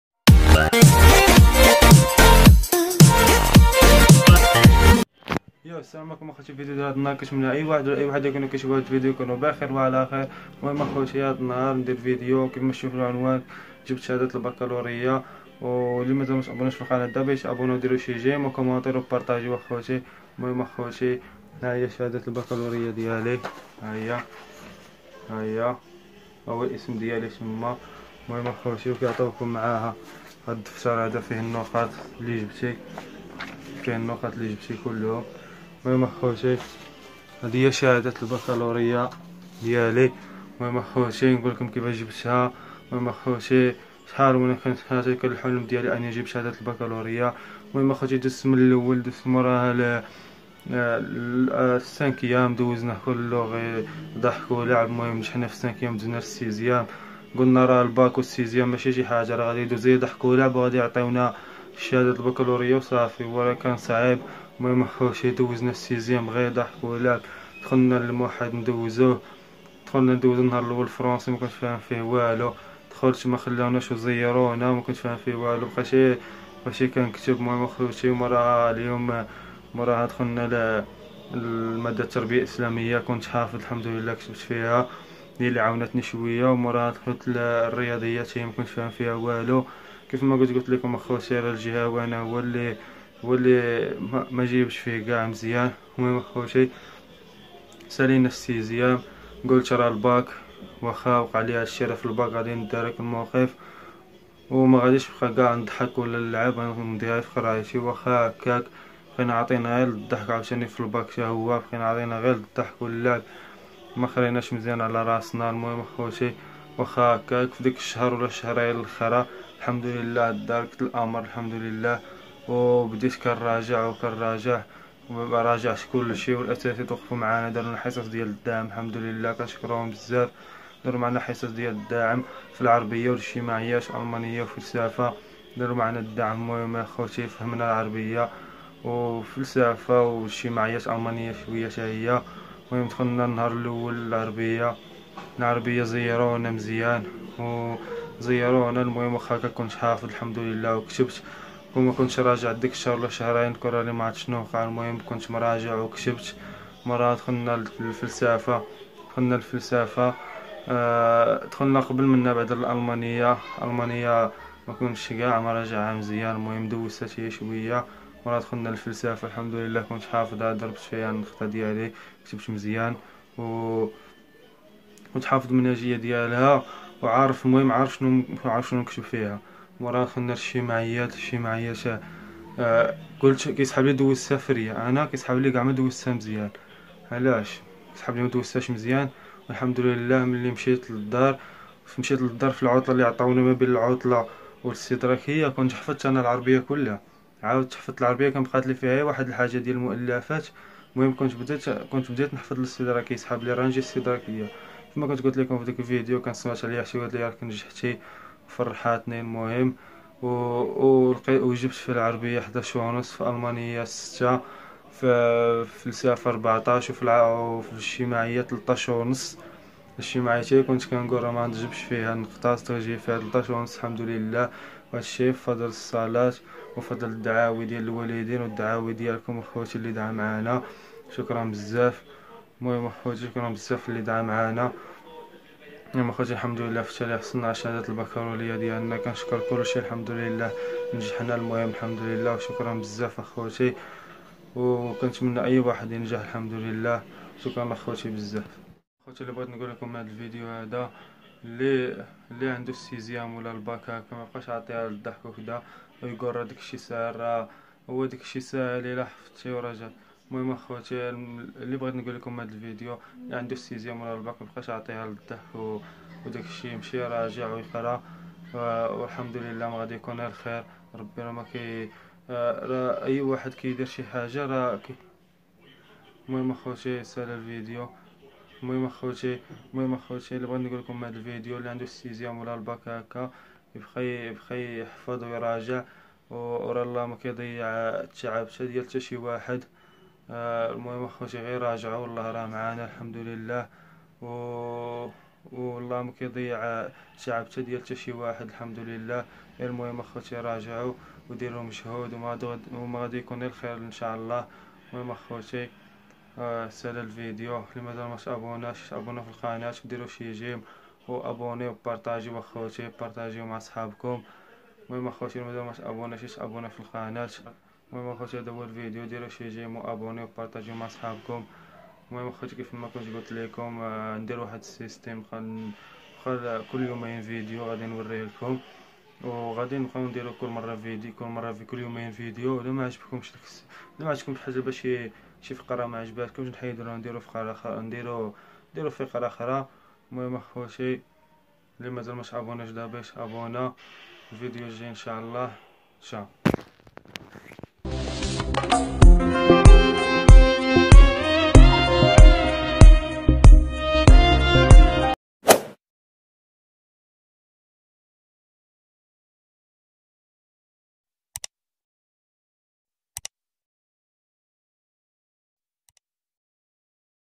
مشيت نجيب شهادة الباكالورية دخلو تشوفو قصتي مع الباكالورية اكيفاش جبتها اشنو دوزت باش خديتها
ديرو جيم اابوني ابرطاجيو اخوتي